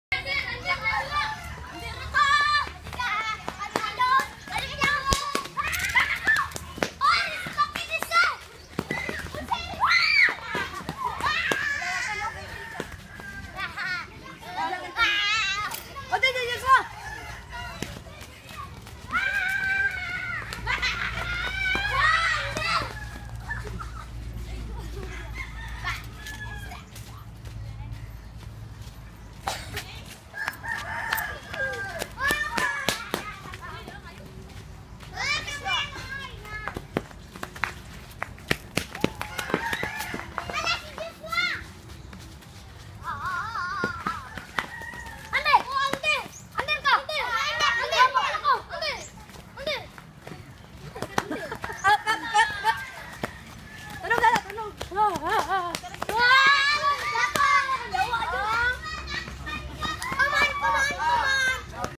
RSP play time activity
The CLSF Emmanuel Toril Mission church team ministers teens and kids of RSP Village. Today since the teens joined TIYF gathering, only few kids played with Ate Karen. They played Ate Karen's ultimate game when she was way too young, "TIG-SO".